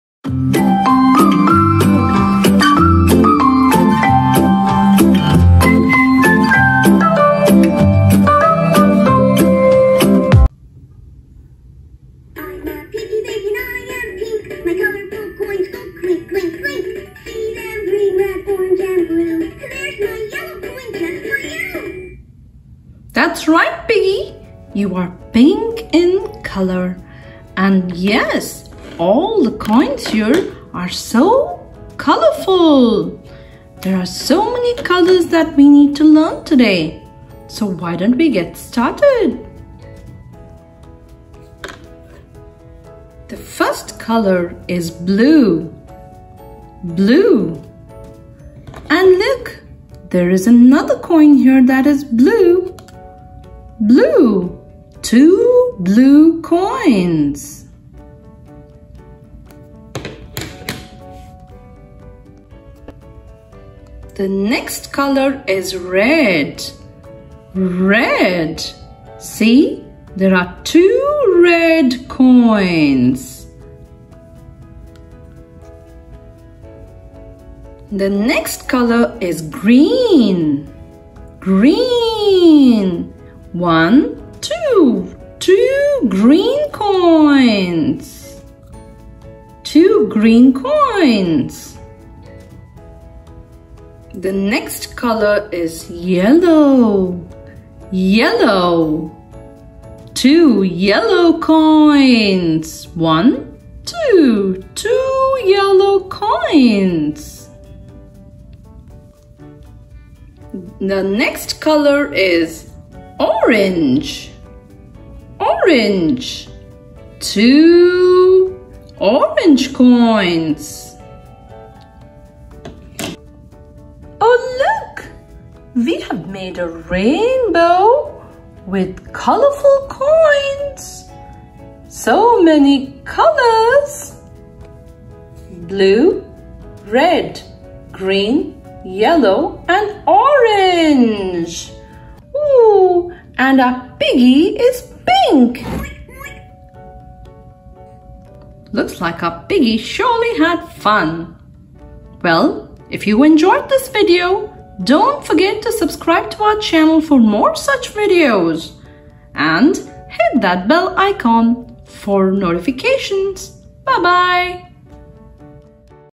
Fun way to learn colors with Fisher price Smart Piggy Bank| Educational video for kids
Fun way to learn colors with  Fisher price Smart Piggy Bank| Best educational video for kids to learn colors